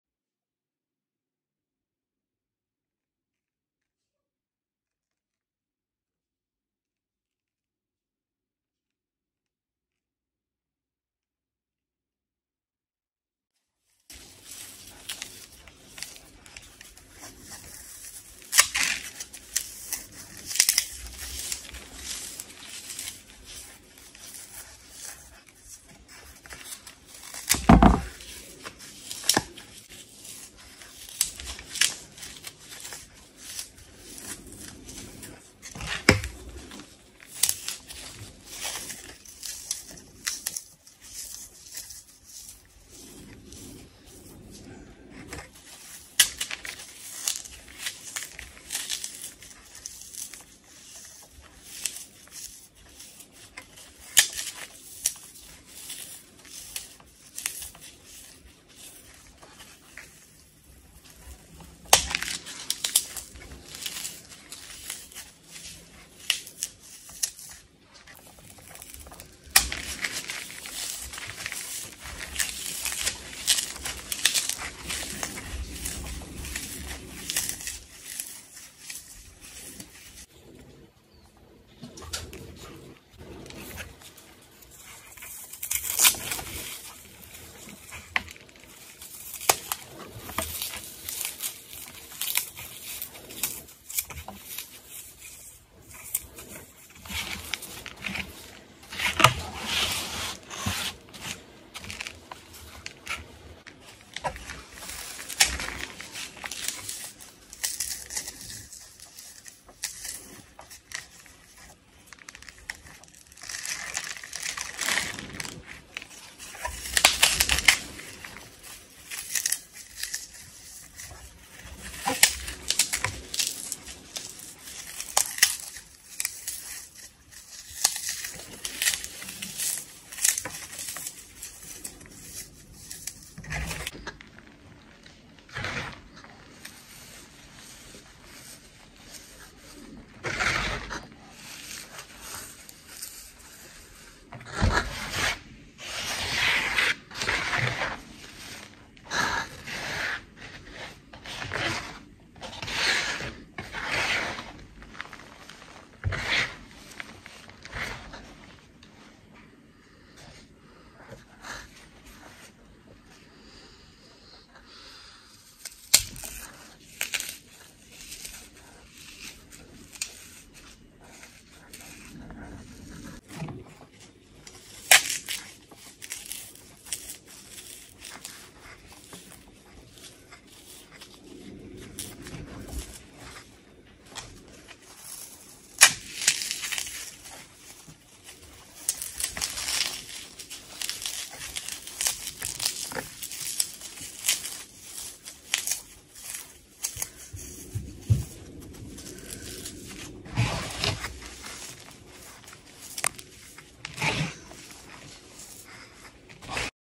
ASMR FLORAL FOAM and GYM CHALK PEBBLES | **requested| CRUNCHY| STRESS RELIEF
Hi everybody! This is a TikTok request, I am really starting to enjoy a different sounds that floral foam makes.

Feel free to always make request, I have so much fun with us.